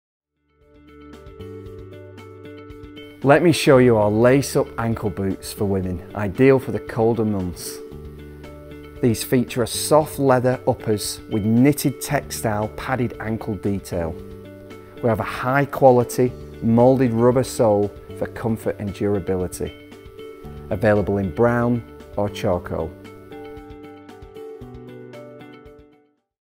High quality ladies suede ankle boots
Featuring a slight wedged heel, these leather ankle boots are ideal for keeping feet feeling warm and looking stylish over the winter months.

Lace-up ankle boots ideal for the colder months
Soft leather uppers with knitted textile padded ankle detail
Rubber wedge sole: 67mm
Available in UK sizes 3 to 8
Available in brown or charcoal